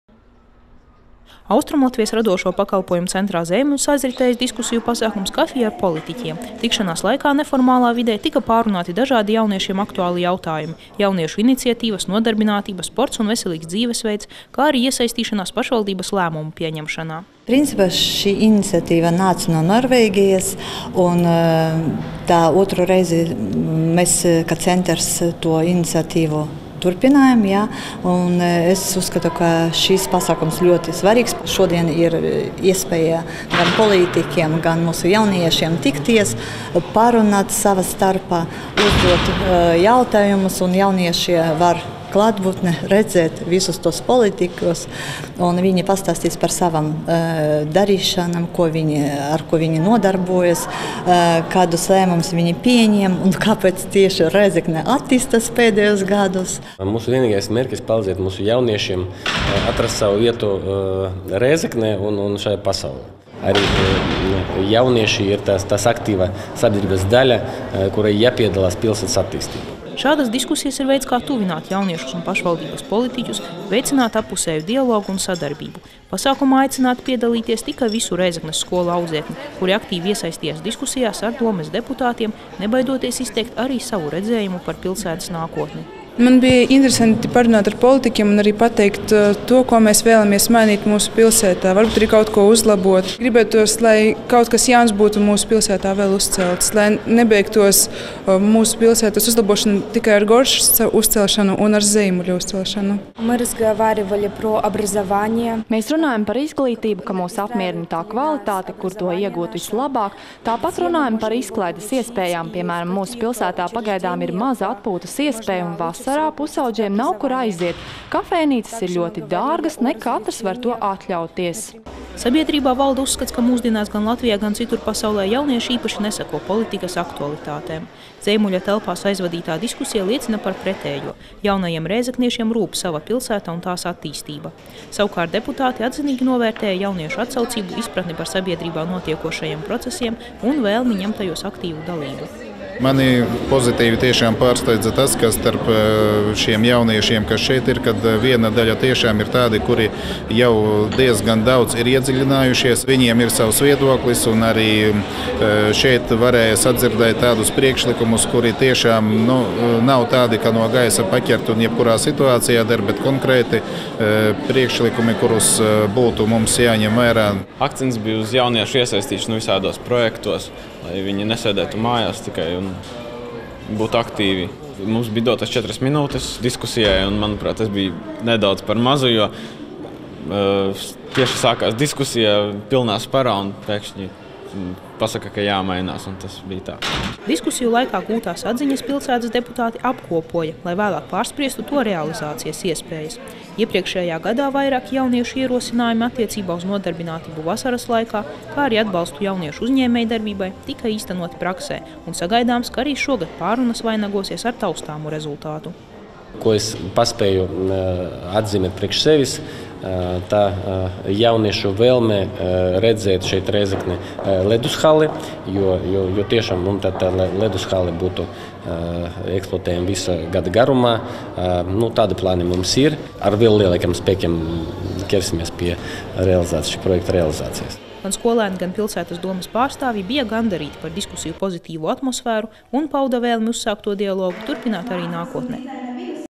Kafija ar politiķiem
“Jaunatne ir mūsu nākotne” - šie vārdi dzirdēti ne vienu vien reizi, taču nereti paši jaunieši atzīst, ka viņu viedoklis un redzējums par sabiedrībā aktuāliem procesiem netiek ņemts vērā. Rēzeknes pilsētas domes deputātiem nu jau par tradīciju kļuvis katru rudeni tikties ar Rēzeknes skolu audzēkņiem, lai iepazīstinātu tos ar politikas procesiem, kā arī smeltos jaunas idejas pilsētas attīstībai.